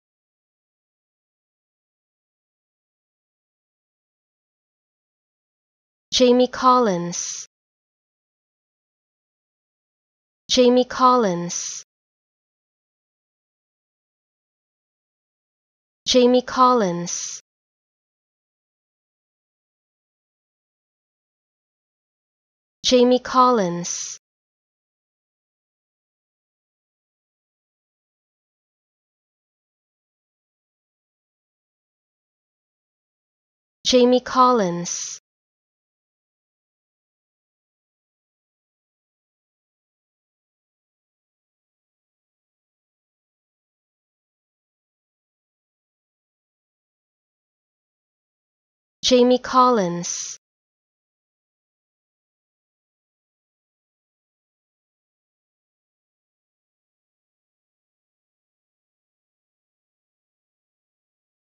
How to Pronounce Jamie Collins
Learn how to say Jamie Collins  with EmmaSaying free pronunciation tutorials.